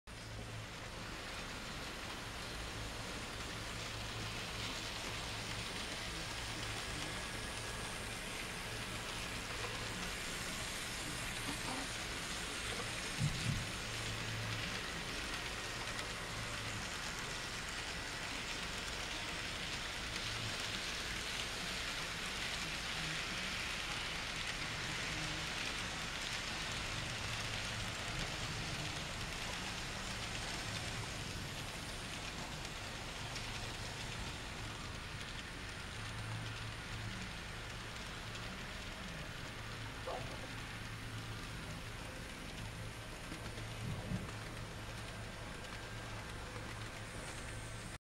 Fox Valley Heritage ES44 NS Conrail and Kato ES44 CP grain train
Both trains meet at the center of town